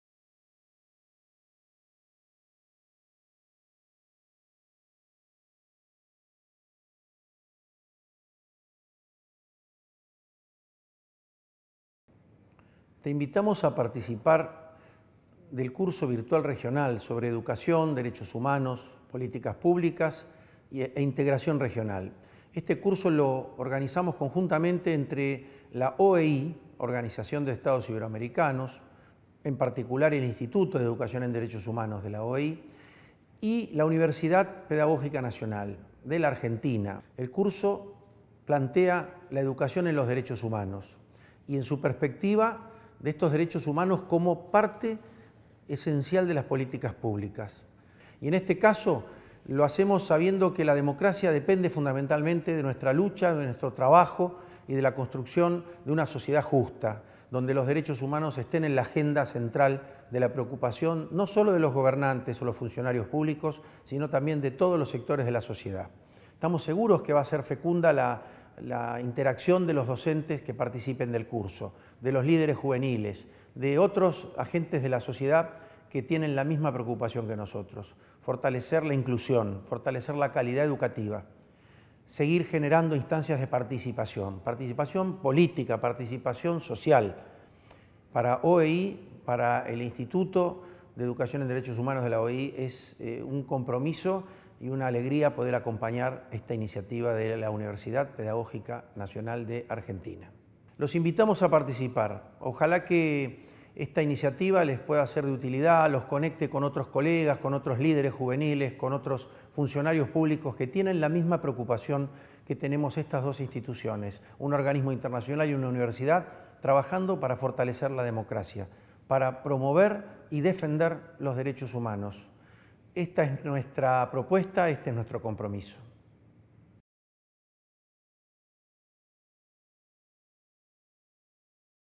Curso virtual Educación y DD.HH. Invitación de Ignacio Hernaiz a participar (organizan UNIPE y OEI)
Invitación del Director de la Oficina de la Organización de Estados Iberoamericanos-Mercosur en Uruguay, Prof. Ignacio Hernaiz, a participar del curso virtual regional "Educación y Derechos Humanos en las Políticas Públicas y la Integración Regional". El curso, de carácter gratuito, y organizado por la Universidad Pedagógica Nacional (UNIPE) de Argentina y por la Coordinación Regional del Instituto Iberoamericano de Educación en Derechos Humanos, Oficina OEI - Mercosur en Uruguay (IDEDH-OEI), recibirá postulaciones hasta el próximo 15 de julio de 2017.

Director de curso: Profesor Norberto Liwski